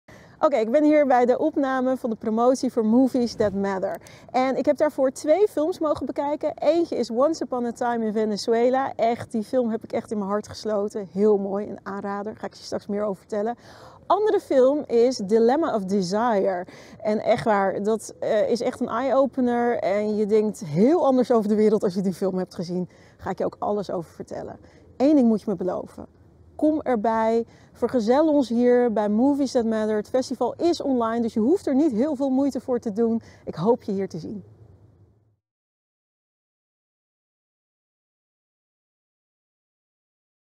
De tips van Natasja Gibbs
📽 Welke festivalfilms je niet mag missen als het aan Natasja Gibbs ligt?

The Dilemma of Desire
On Demand van vr 16 april 20:00 - di 20 april 04:00

De clitoris is het enige lichaamsdeel uitsluitend bedoeld voor genot. Maar ondanks die eenduidige functie hebben veel mensen geen idee hoe ze werkt of eruit ziet. The Dilemma of Desire verandert dat voor eens en altijd. Een warm en fascinerend pleidooi voor seksuele vrijheid en kennis.

Once Upon a Time in Venezuela
On Demand van zo 18 april 17:00 - do 22 april 01:00

Tamara is een corrupte afgevaardigde van de heersende socialistische partij, Natalie een lokale onderwijzeres en aanhanger van de oppositie. Gefilmd over meerdere jaren, wordt de Venezolaanse politiek weerspiegeld in een kleine drijvende nederzetting in het meer van Maracaibo, met adembenemende beelden van het meer, het dorp en zijn veerkrachtige inwoners.

Beide films zijn voorzien van een korte inleiding door Natasja.